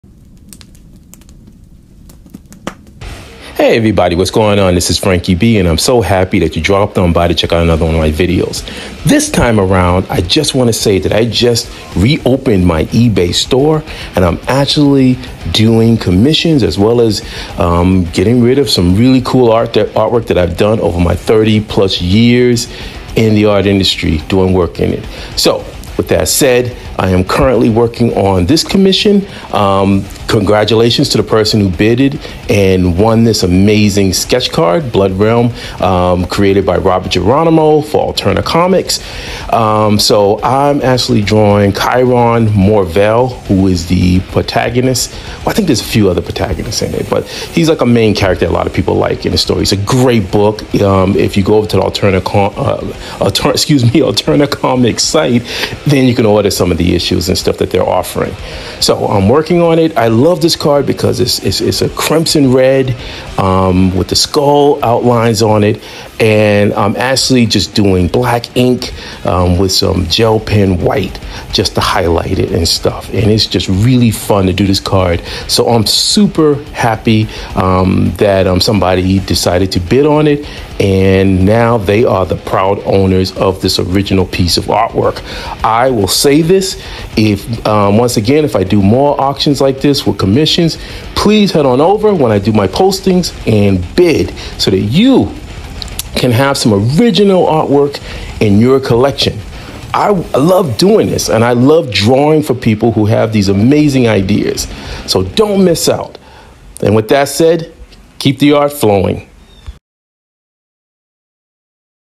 BloodRealm Sketch Card Commission
COMMISSION UPDATE!!! One Down & Three More To Do. I’ve drawn Rob Geronimo’s characters from Blood Realm  for a number of commissions and it never gets old. I still have ART available.